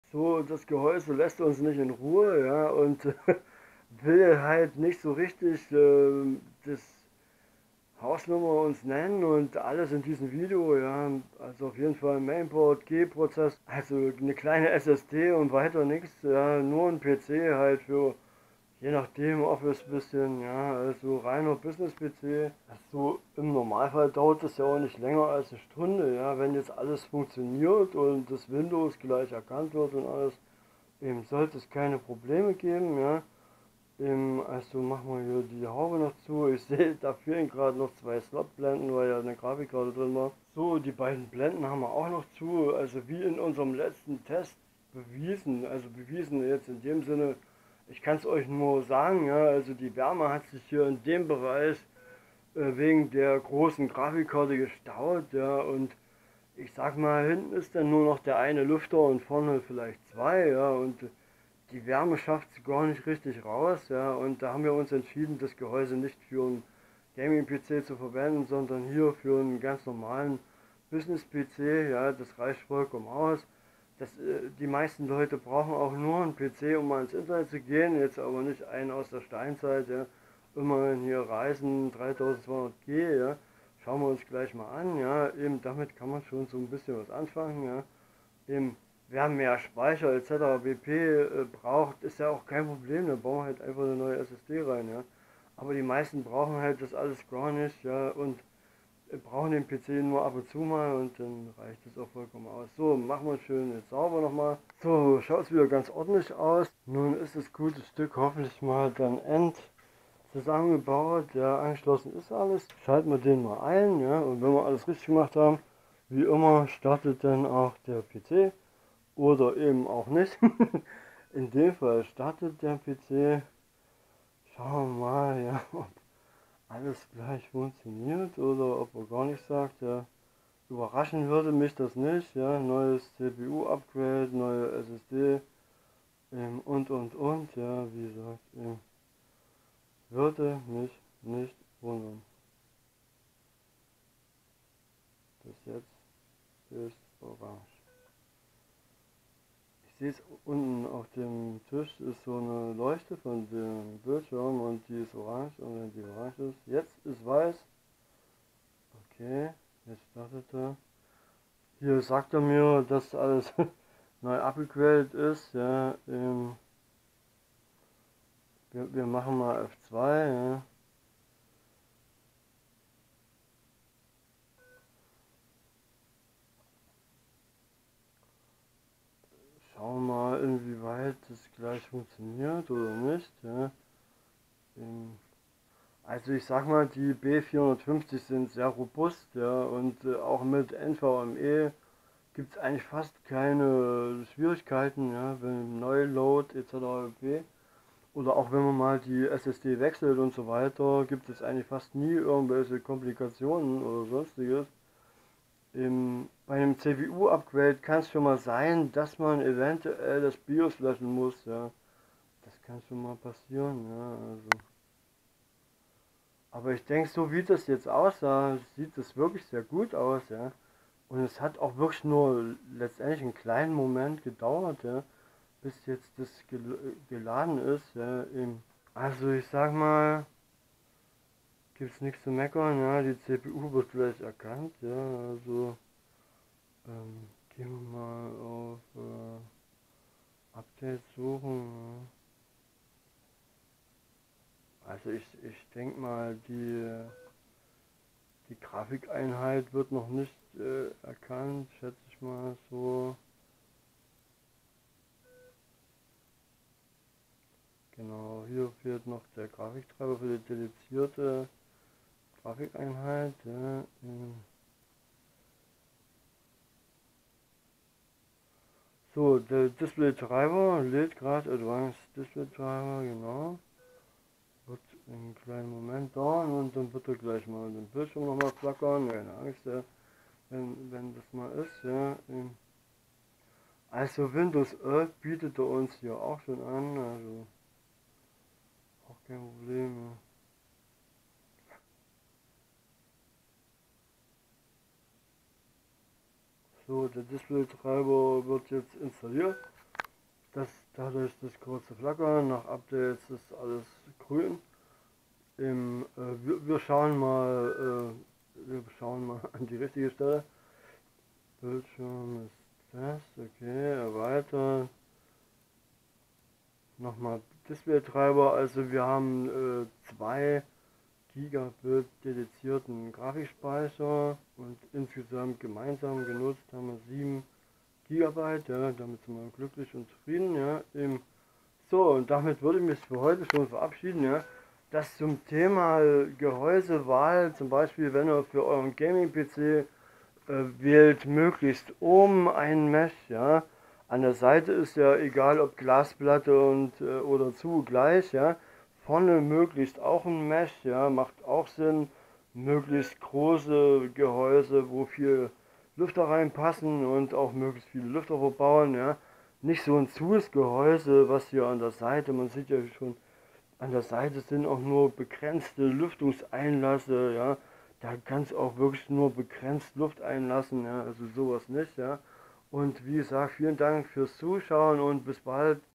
Business PC die Richtige Wahl des PC Gehäuses
Heute, Business PC eindeutig ein Business Gehäuse, die Grafikkarte die drin Verbaut gewesen ist hat eine hohe Wärmeentwicklung das hatte zur Folge das System schnell überhitzt Wurde. Wir hatten den PC im Internet gekauft es hatte zwei Lüfter und eine 20 Terra Flop Grafikkarte,  in kälteren Regionen wahrscheinlich okay, aber in Mitteleuropa wird es im Sommer zum Alptraum.